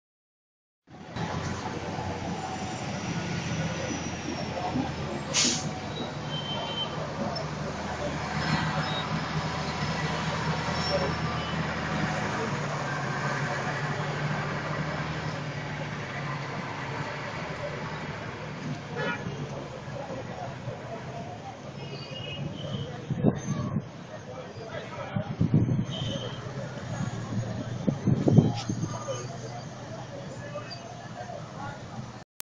Intersection Sroked Impression
For best viewing experience watch in full screen mode. A Digital Video painting (DVP) sample. A stroked impression rendering of a busy intersection.